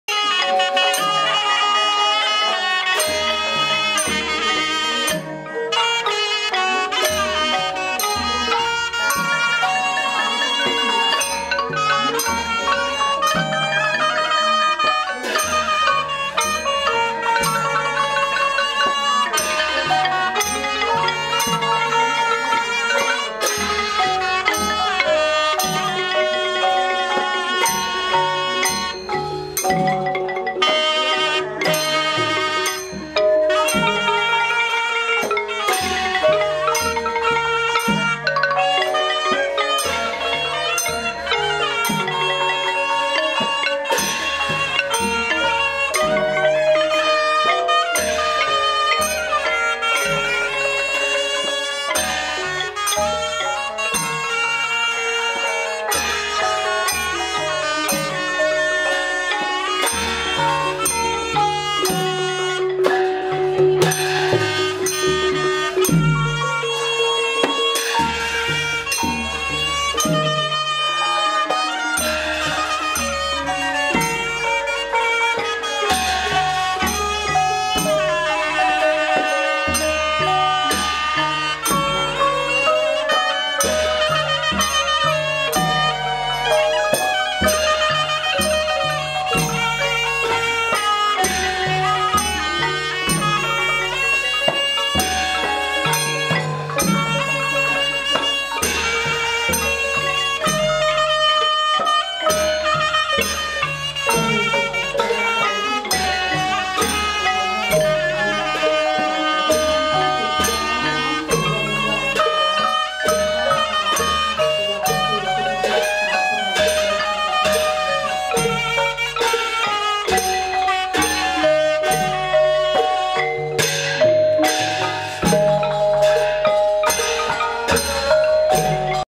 พิธีพระราชทานเพลิงศพ.. พ่อถนอม ข่ายสุวรรณ
พิธีพระราชทานเพลิงศพ.. พ่อถนอม ข่ายสุวรรณ ศืริอายุ 83 ปี ข้าราชการบำนาญ กรมการพัฒนาชุมชน กระทรวงมหาดไทย เมื่อวันอาทิตย์ที่ 7 สค.2565 ณ วัดป่าแดด ต.ท่าวังทอง อ.เมือง จ.เชียงราย...